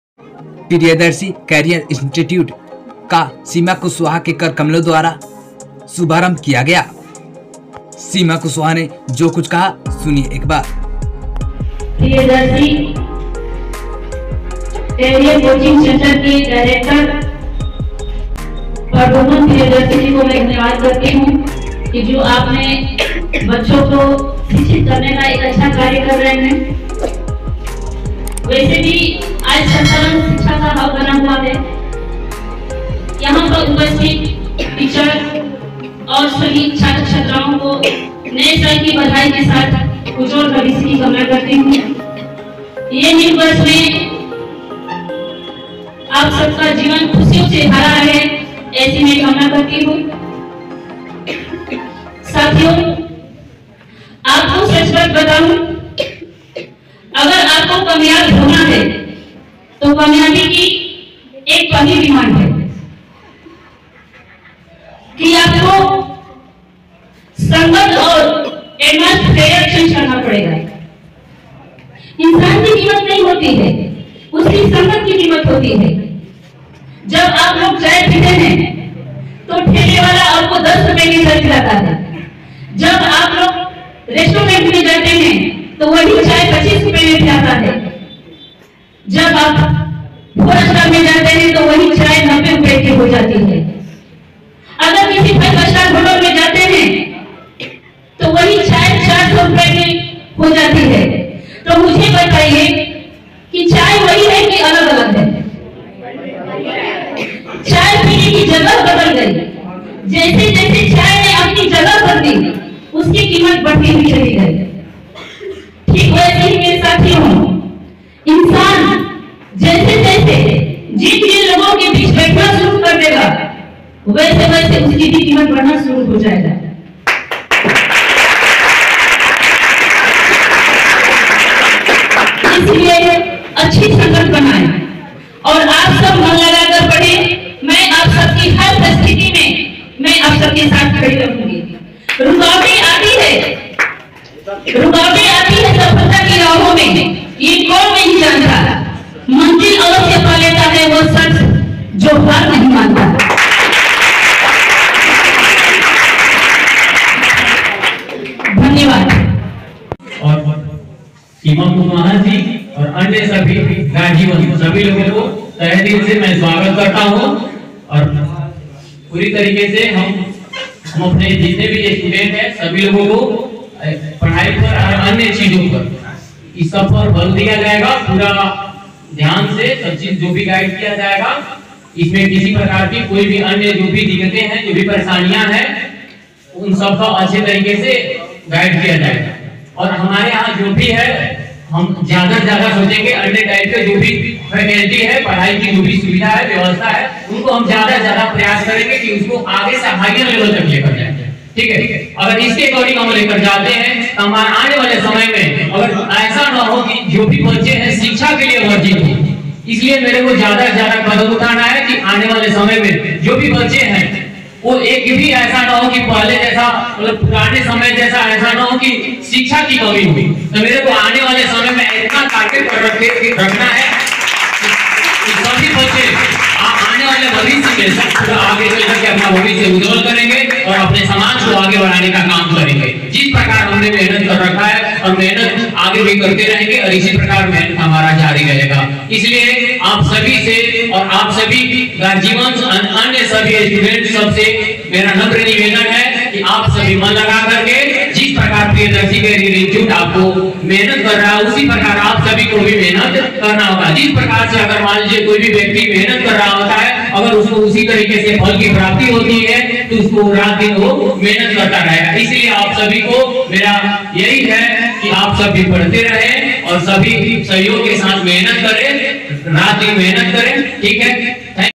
प्रियदर्शी कोचिंग सेंटर का उद्घाटन करने पहुंचे सीमा कुशवाह@MEDIAEXPRESSNEWS
प्रियदर्शी कोचिंग सेंटर का उद्घाटन करने पहुंचे सीमा कुशवाह‎@MEDIAEXPRESSBIHAR